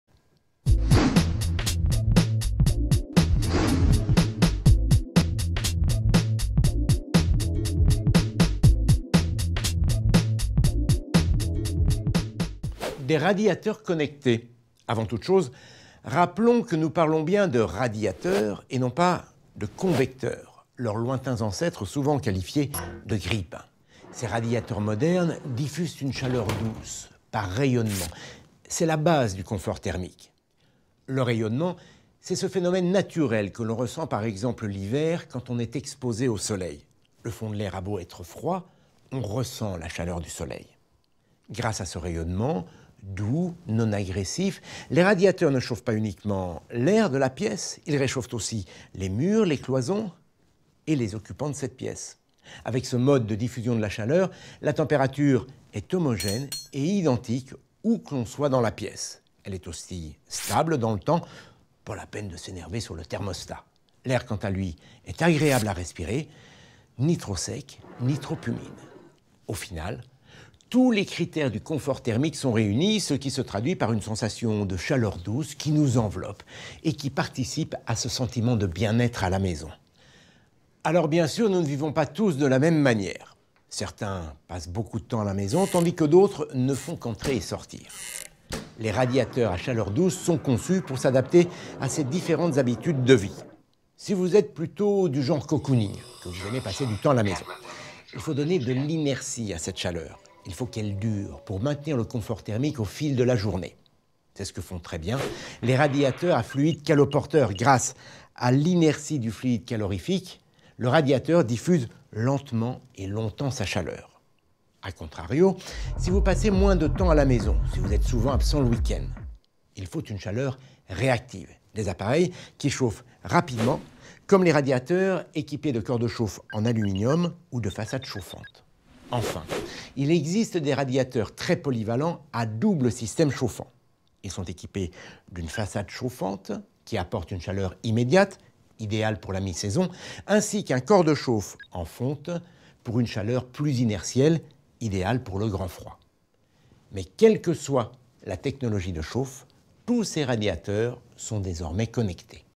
Radiateurs électriques connectés - Le confort d'un radiateur électrique chaleur douce - Thermor
▶️ Qu'est-ce qu'un radiateur électrique connecté ?
On parle souvent de radiateurs connectés pour en réalité faire référence aux radiateurs intelligents. Il s’agit bien de deux types d’appareils différents, bien que les modèles connectés soient souvent également intelligents.

Un radiateur connecté est un appareil de chauffage qui offre la possibilité d’un pilotage à distance via son smartphone ou sa tablette. La plupart du temps, ce type de radiateur est aussi capable d’entrer en liaison avec les autres radiateurs du logement. Ainsi, par exemple, si un radiateur connecté capte la présence d’une fenêtre ouverte, il va se mettre en veille hors gel et « prévenir » les autres radiateurs de la pièce de faire de même.

Un radiateur intelligent, quant à lui, comporte des fonctionnalités dites « intelligentes » telles que :
- La détection de fenêtre ouverte ou fermée ;
- L’adaptabilité aux habitudes des occupants du logement ;
- L’anticipation de la chauffe.

Mais le radiateur intelligent n’est pas forcément un appareil connecté !

▶️ Qu'est-ce qu'un radiateur électrique chaleur douce ?
Il s’agit d’un radiateur électrique embarquant les dernières avancées technologiques pour vous faire profiter d’un confort thermique optimal. Lorsqu’un convecteur électrique traditionnel fonctionne par à-coups de chaleur intense et source de sensation chaud/froid un radiateur chaleur douce diffuse une chaleur en continue, homogène et agréable dans toute la pièce.

Terminés les écarts de température entre le sol et le plafond ! Dans votre intérieur règne une chaleur douce et enveloppante.

Il existe ainsi deux grandes familles de radiateurs électrique à chaleur douce : le modèle à inertie sèche et celui à fluide caloporteur.

00:00 Introduction
00:12 Le radiateur connecté
00:24 Les radiateurs modernes
01:20 Les habitudes de vie